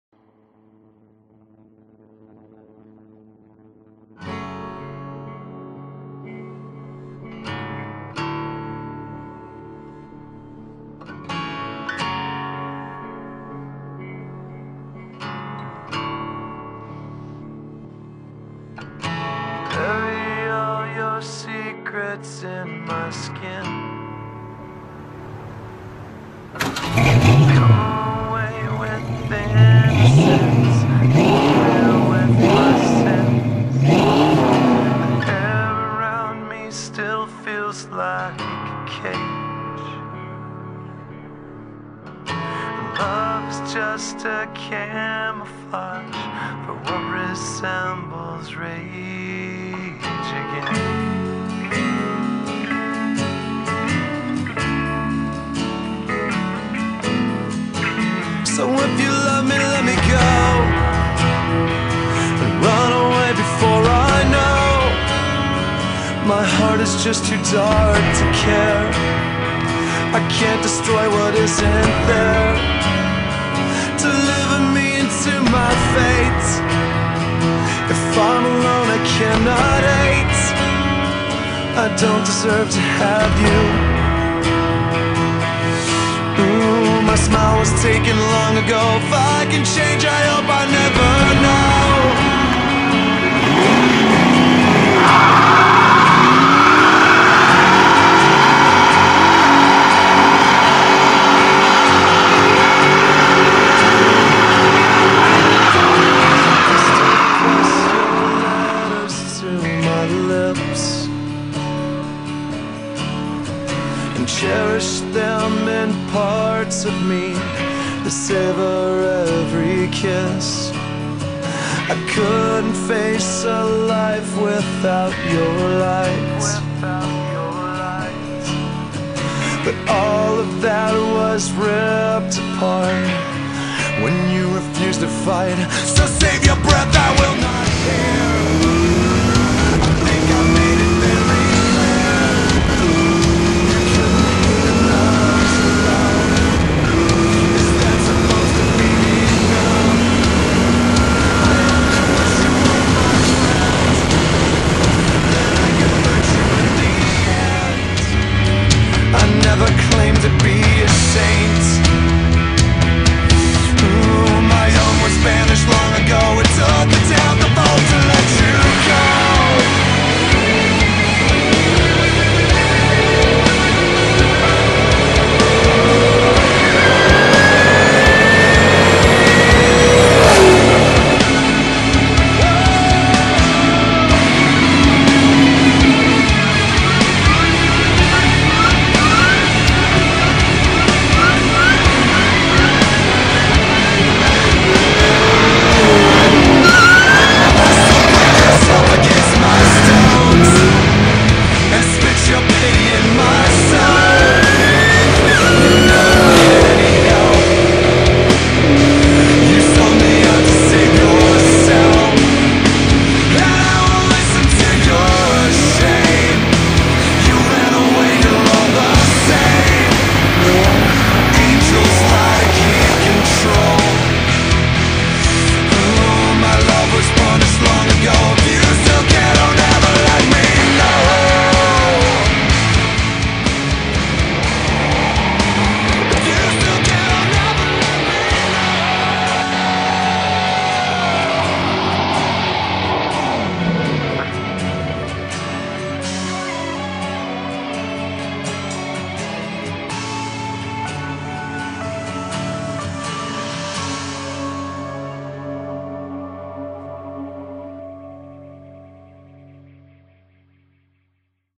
Ford Mustang Compilation
Video showing off some of my favorite Mustang's over the years...